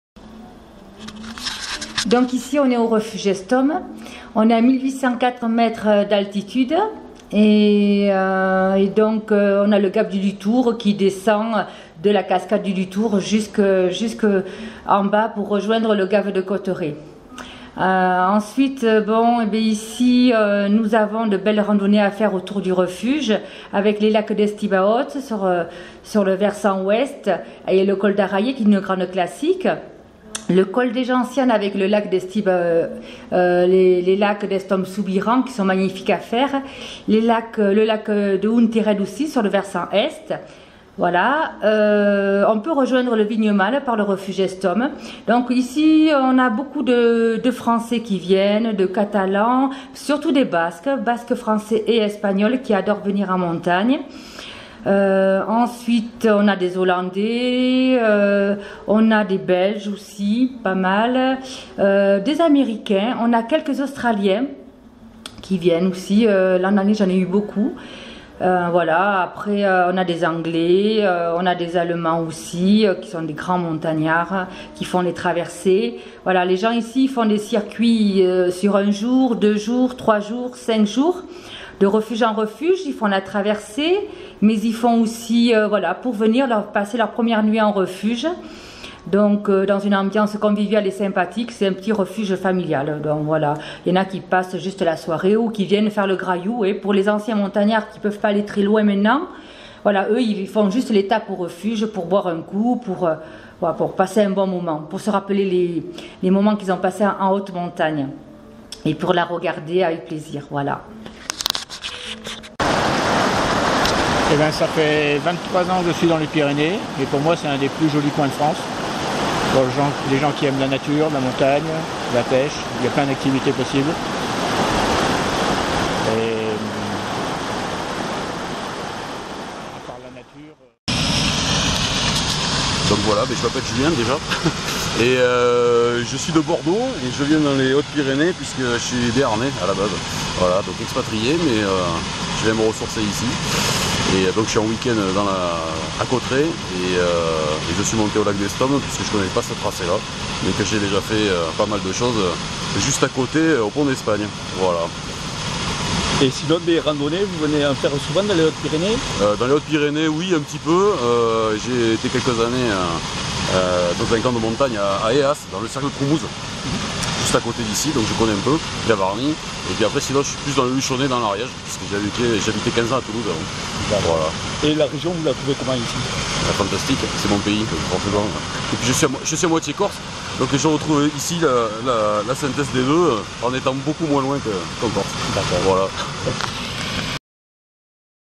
lac d'estom
petite randonnée de  4 heures aller , retour  au lac d'estom cauterets hautes pyrénées vallée du lutour